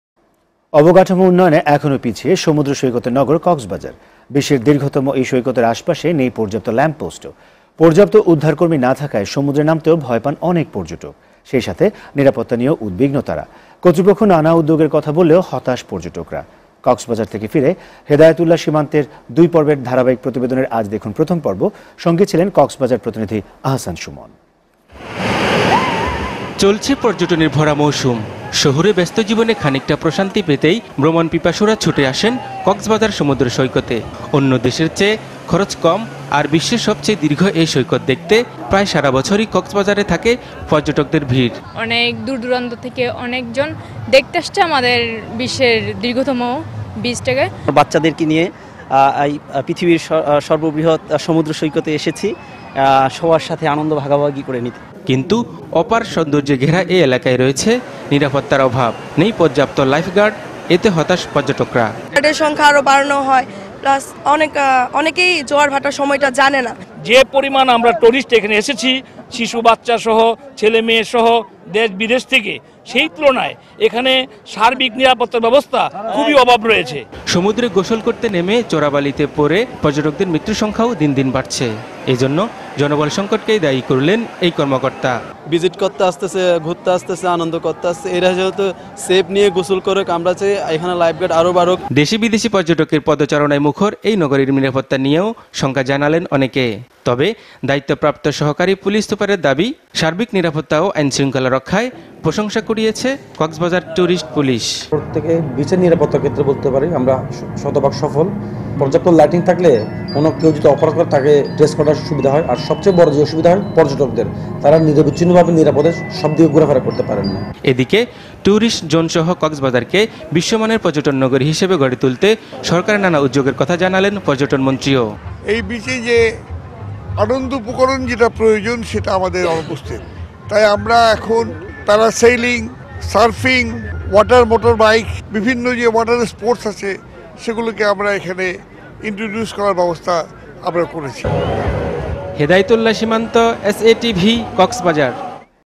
SATV NEWS COXBAZAR 01_08 JAN 2016
Report: Tourism Industry of Cox'sbazar
Reporter: Hadayet Ullah Simanto